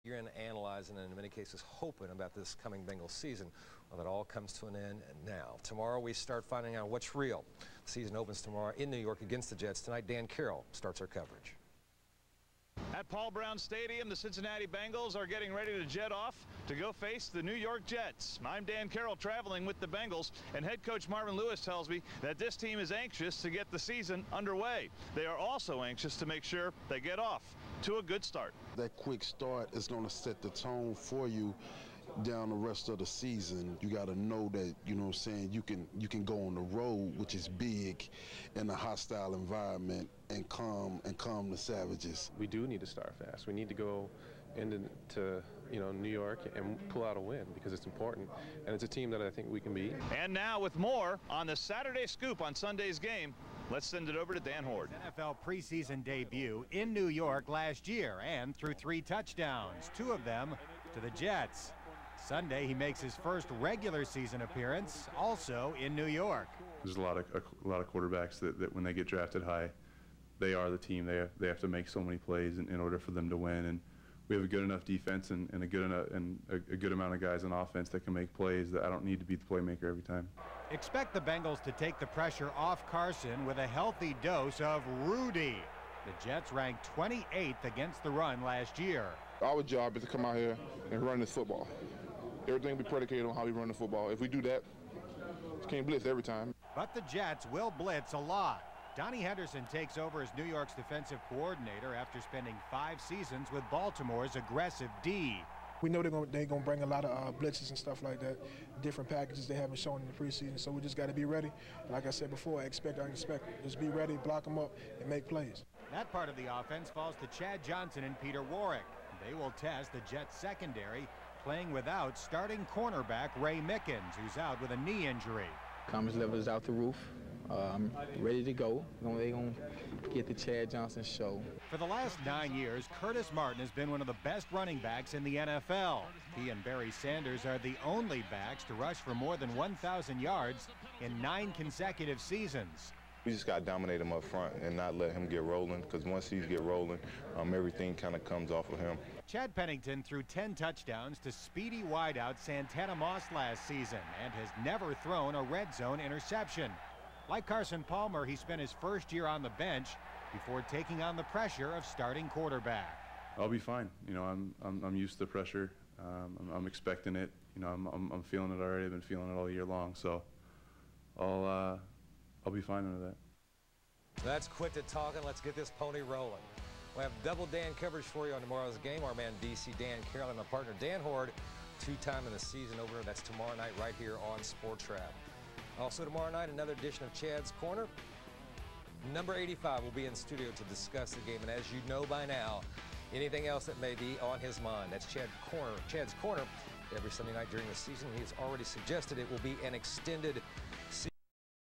2004 Bengals-Jets Week 1 preview (Fox 19 Cincinnati)
Fox 19 previews the Week 1 matchup between the Bengals and the Jets. Interviews with Carson Palmer, Bobbie Williams, Kevin Kaesviharn, Willie Anderson, Rudi Johnson, Brian Simmons, and Chad Johnson